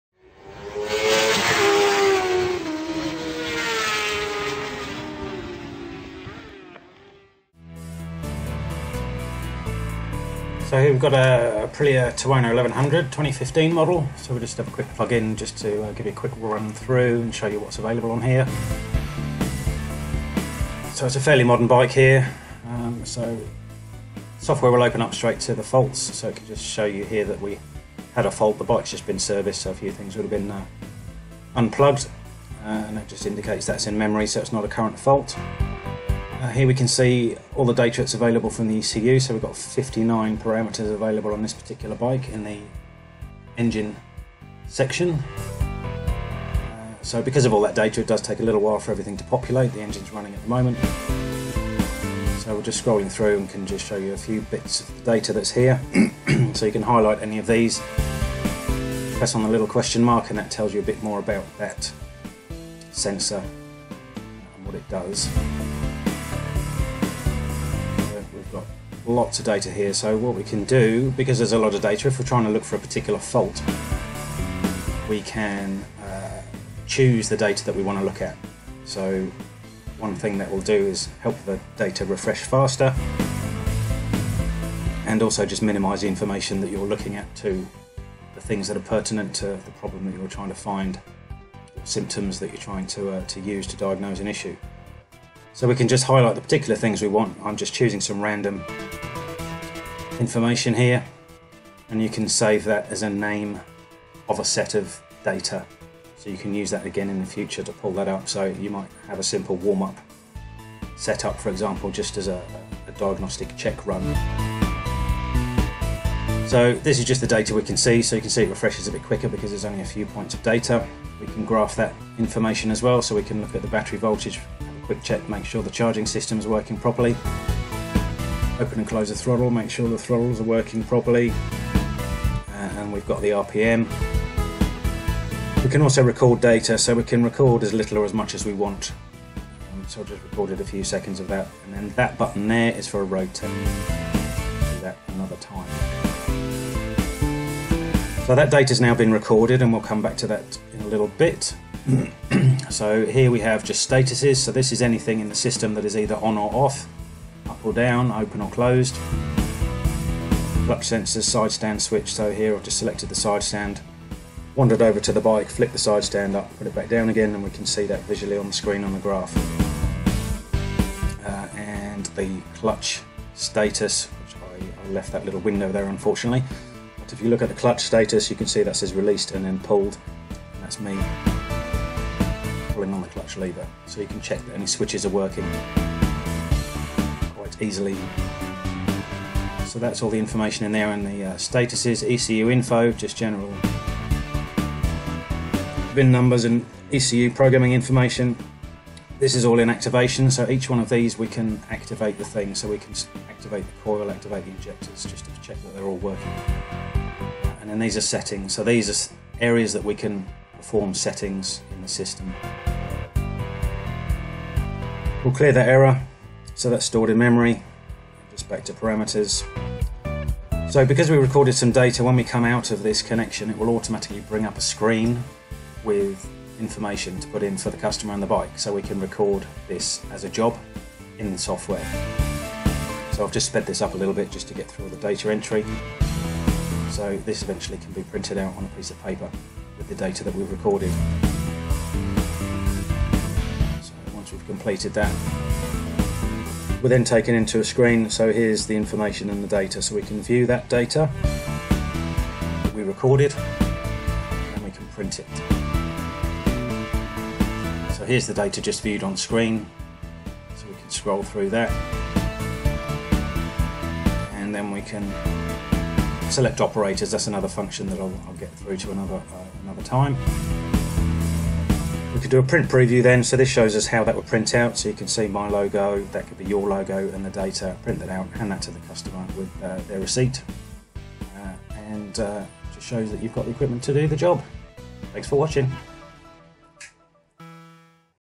Aprilia Tuono 1100 2015. Demo using Texa Navigator TXB Evolution with IDC5 Plus Bike software.
While at a shop a 2015 Tuono was there so took the chance to plug in and get some data to record while showing you a quick run through of some features that people may not be aware of - Data descriptions, Data filter, Data recording, Customer management, Job information storing and Data printing.